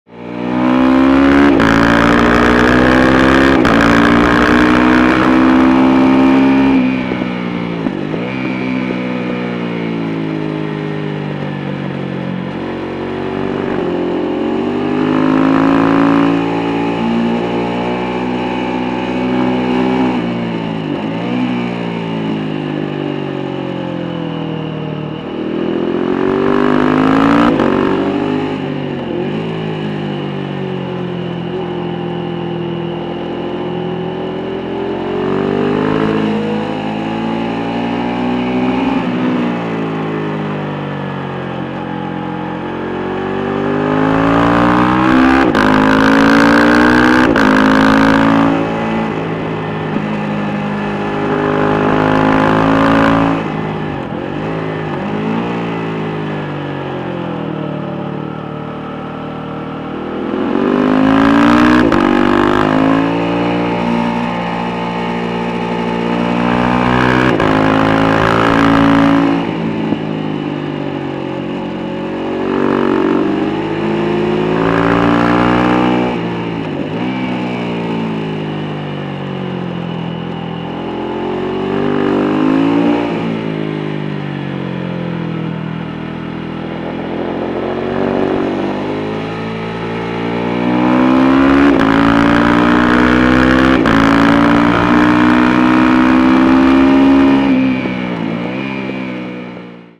KTM 690 Duke R + Healtech iQSE / iLogger Easy - Track Day (RAW sound)
KTM 690 Duke R '16:
- Healtech Quick Shifter easy (iQSE)
- Healtech iLogger easy
- Akrapovic slip-on + decat